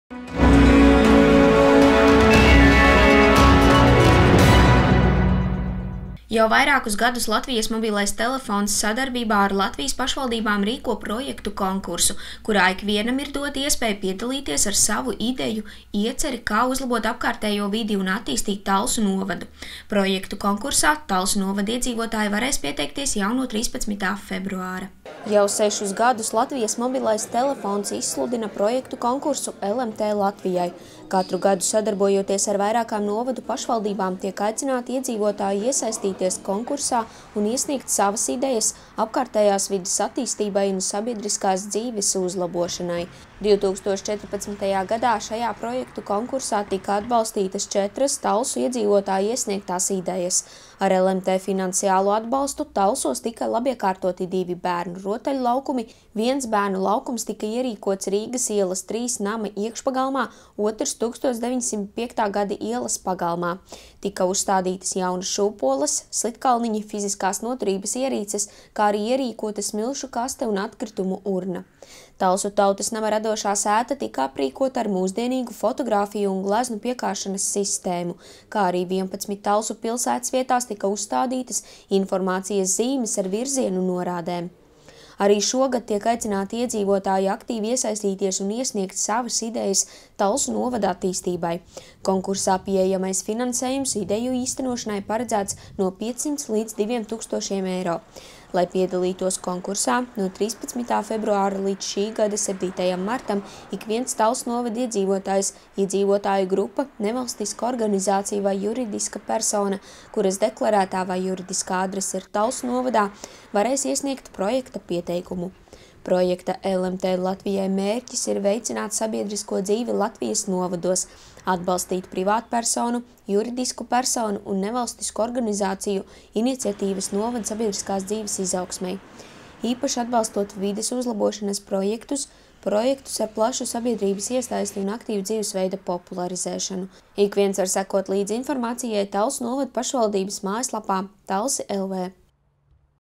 Būs iespēja piedalīties projektu konkursā “LMT Latvijai”
Jau vairākus gadus Latvijas Mobilais Telefons sadarbībā ar Latvijas pašvaldībām rīko projektu konkursu, kurā ikvienam ir dota iespēja piedalīties ar savu ideju, ieceri, kā uzlabot apkārtējo vidi un attīstīt Talsu novadu. Projektu konkursā Talsu novada iedzīvotāji varēs pieteikties jau no 13.februāra.